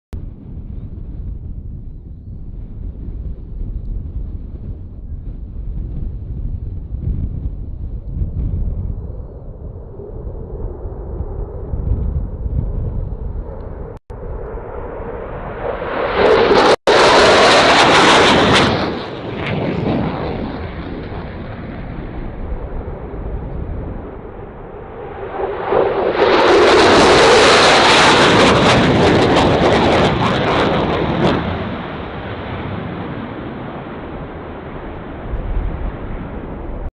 USAF F-15 strike eagles in the mach loop
check out the full video of F-15 and F-35 low level in the machloop north wales